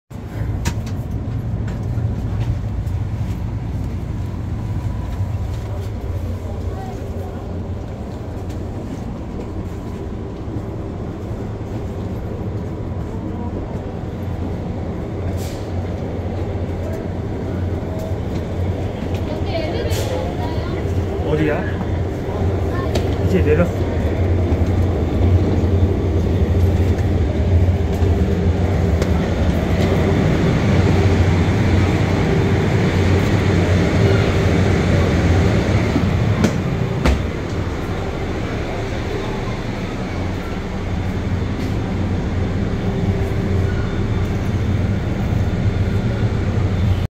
[4K 60fps] 20230519 133405 금정역에 대적하는 순천역 5초 환승
무궁화 1941 부전발 순천행 종착
6분 후
무궁화 1973 순천발 광주송정행 시발
같은 승강장 취급
동부경전선과 서부경전선 릴레이 연계

촬영 : Samsung Galaxy S 22 Ultra (SM-S908N)